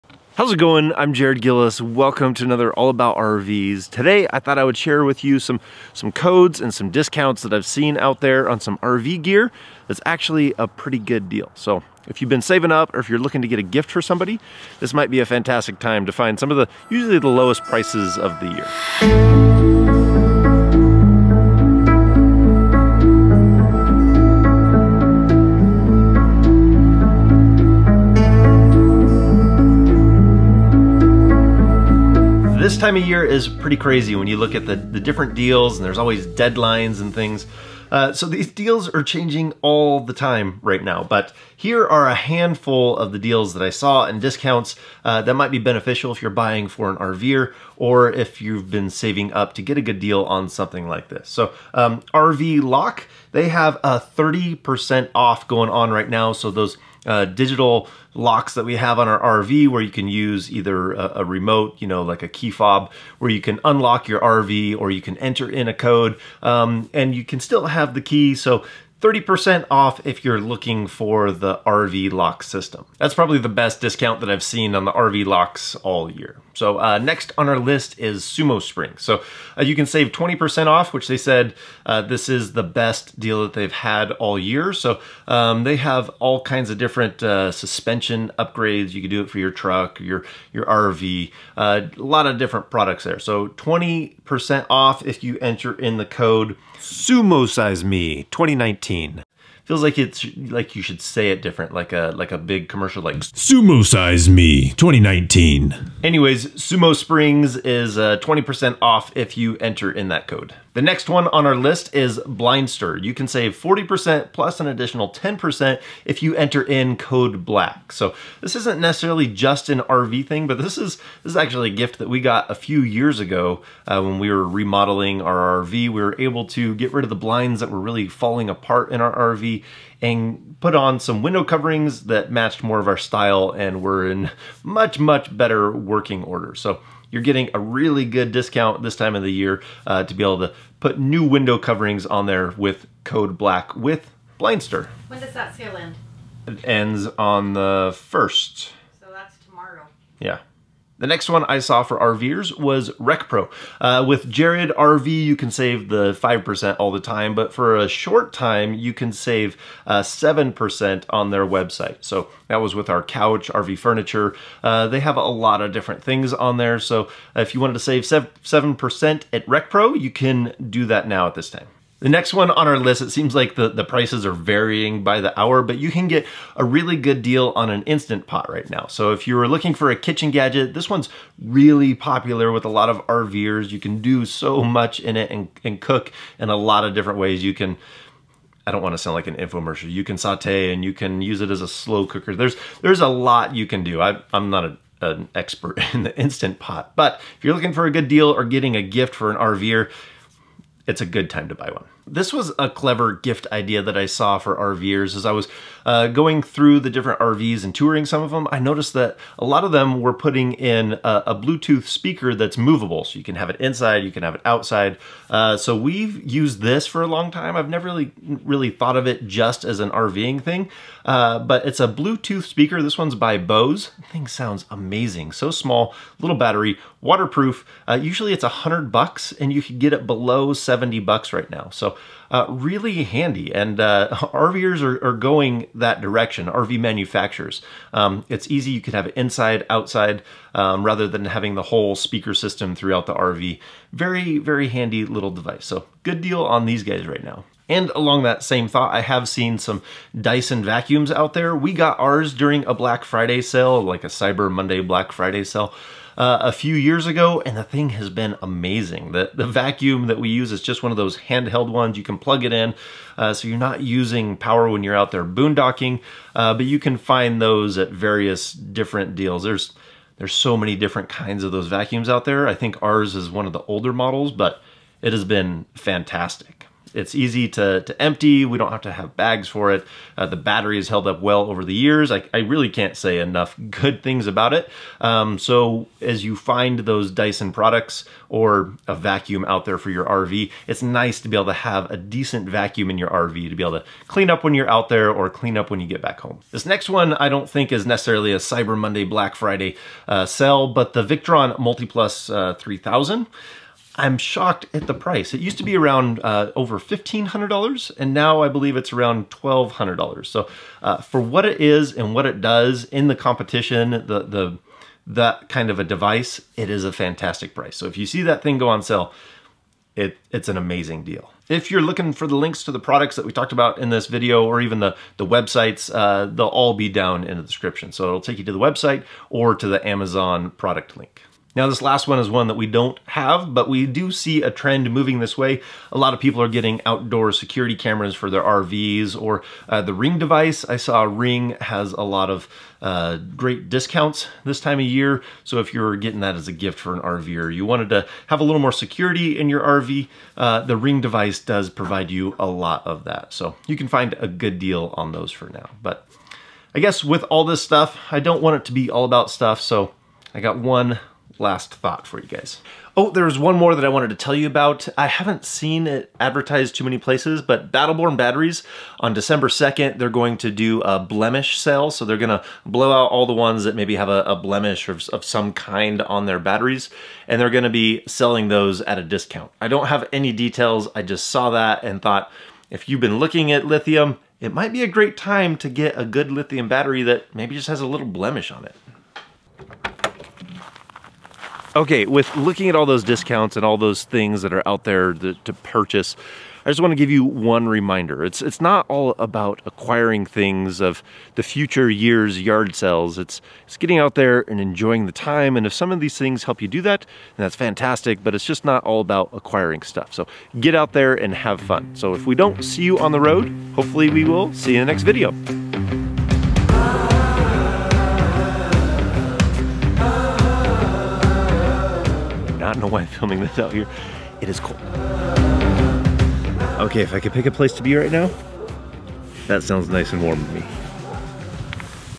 RV Gear Deals 2019!
Here a few RV gear deals we have come across that are better than your average discount!

Sumo Springs amazon.
Bose Micro Bluetooth.
Dyson Vacuum
Victron MultiPlus.

Here is our Amazon page for our favorite gear. We will be adding more products as we find more products we use and love.